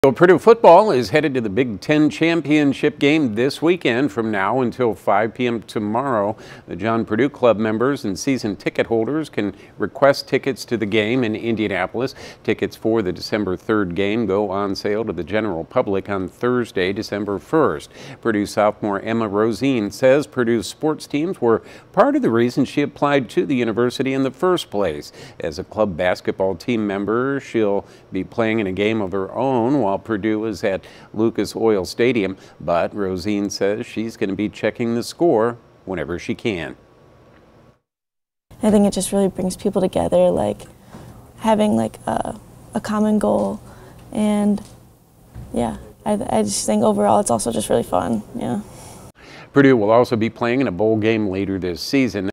"It may never happen again," students react to Purdue Football's Big Ten Championship chance
Thursday Dec. 1 tickets for Purdue's Big Ten championship game go on sale to the general public.